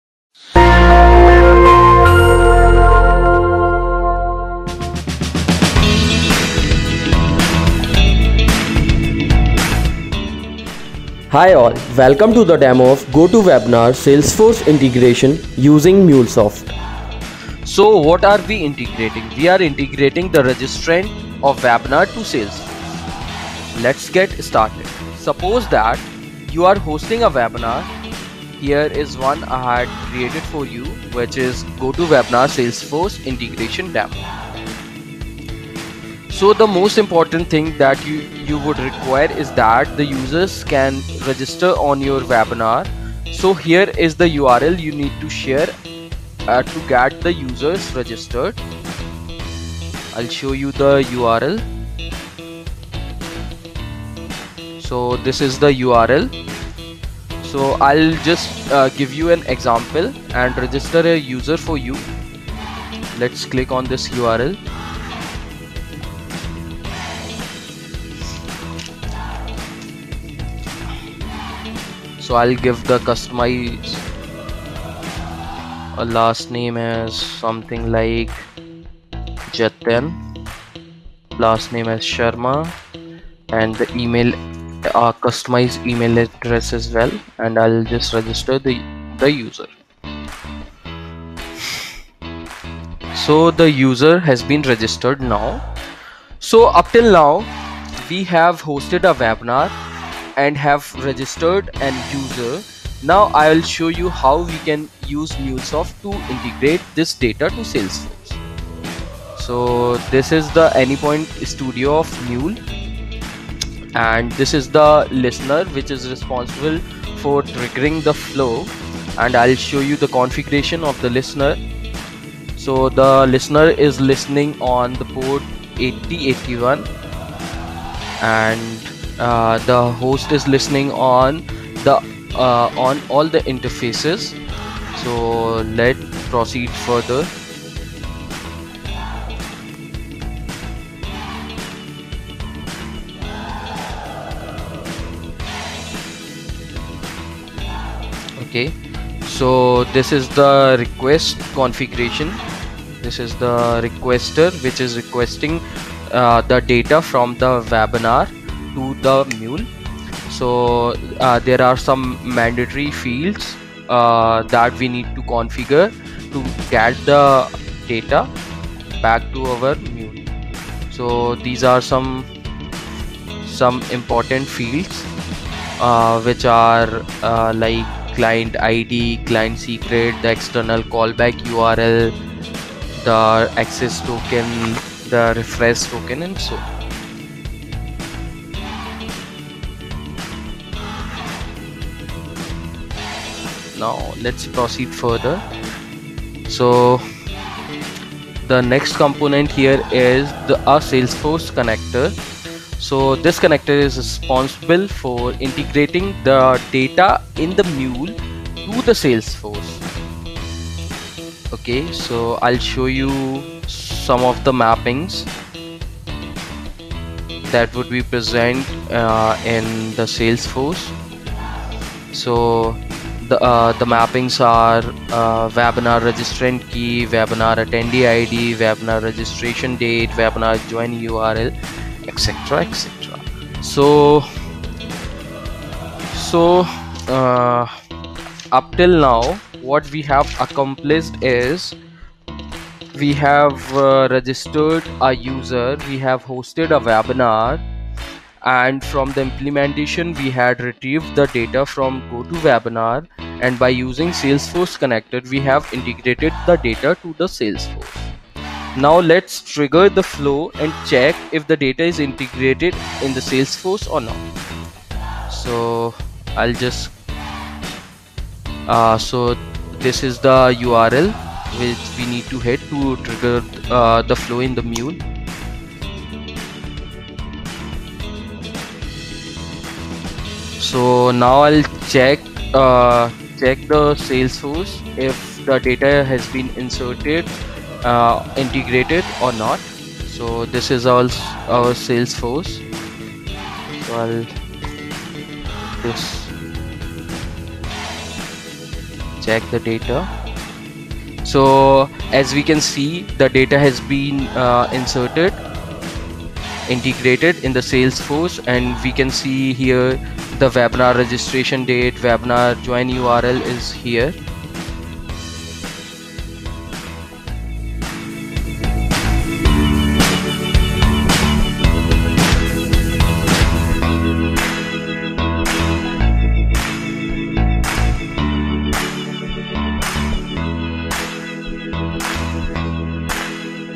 Ep 2 - MuleSoft Integration: GoToWebinar to Salesforce - Learn Salesforce Series with Algoworks
In this video, you will learn how to use MuleSoft integration platform to transfer attendees data from GoToWebinar to Salesforce-
1. Set up GoToWebinar account, create a new webinar and register a user to this webinar.
2. Create a MuleSoft job and configure HTTP connector to access GotoWebinar data.
3. Complete the Salesforce connector configuration in the same MuleSoft job.
4. Execute MuleSoft job to migrate GoToWebinar attendees data to Salesforce.

Hi all. Welcome to the demo of GoToWebinar Salesforce integration using MuleSoft.  So what are integrating? We are integrating the registrants of Webinar to Salesforce. So let’s get started.

Suppose that you are hosting a webinar. Here is one I have created for you, which is GoToWebinar Salesforce integration demo. So the most important thing that you would require is that the users can register on your webinar. So here is the URL you need to share to get the users registered. I will show you the URL. So this is the URL. So I will just give an example and register a user for you. Let’s click on this URL. So I will give the customised first name as something like ‘Jatin’, last name as ‘Sharma’. So the user has been registered now. So, up till now we have hosted a webinar and have registered a user. Now I will show you how can use MuleSoft to integrate this data to Salesforce.

So this is the any point studio of Mule and this is the ‘Listener’ which is responsible for triggering the flow and I will show you the configuration of the listener. So the listener is listening on the ‘Port 8081’ and the host is listening on all the interfaces. So let’s proceed further. Hey. So this is the ‘Request’ configuration. This is the ‘Requestor’ which is requesting the data from the webinar through the Mule. So there are some mandatory fields that we need to configure to get the data back to our mule. So these are some important fields which are like ‘Client ID’, ‘Client secret’, ‘External callback URL’, the ‘Access token’, the ‘Refresh token’ and so on.

Now let’s proceed further. So the next component here is the ‘Salesforce connector’. So this connector is responsible for integrating the data in the Mule with Salesforce. Ok, so I will show you some of the mappings that would be present in the Salesforce. So the mappings are’ Webinar registrant key’, ‘Webinar attendee ID’, ‘Webinar registration date’, ‘Webinar join URL’, etc etc. So, up till now what we have accomplished is we have registered a user, we have hosted a webinar and from the implementation we had retrieved the data from GoToWebinar and by using Salesforce connector we have integrated the data to the Salesforce.

So now lets trigger the flow and check if the data is integrated in the Salesforce or not. So this is the URL which we need to hit to trigger the flow in the Mule. So now I will check the Salesforce to check if the data has been inserted, integrated or not. So this is all our Salesforce. I will just check the data. So as we can see the data has been inserted, integrated in the Salesforce. We can see here the ‘Webinar registration date’, ‘Webinar join URL’ is here.